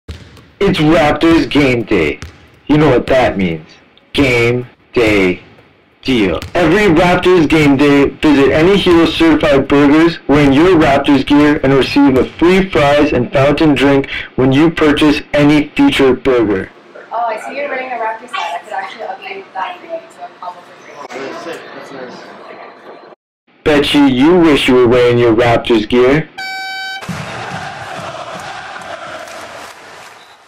Raptors GameDayDeal at Hero Certified Burgers
Complete Your Meal with the GameDayDeal.

Wear any Raptors gear on Game Day and receive Free Fries & Fountain Drink with purchase of a Feature Burger.